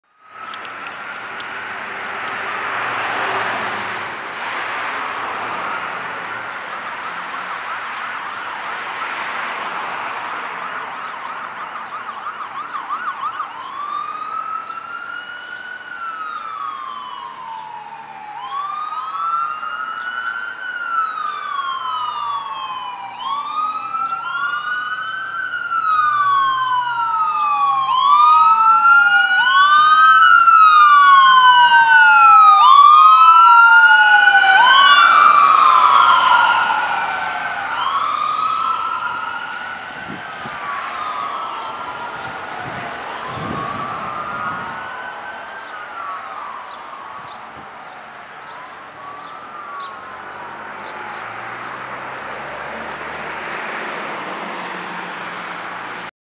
Lutheran EMS Responding
Captured in this video is Lutheran EMS Medic 31 responding to a medical call in Peru.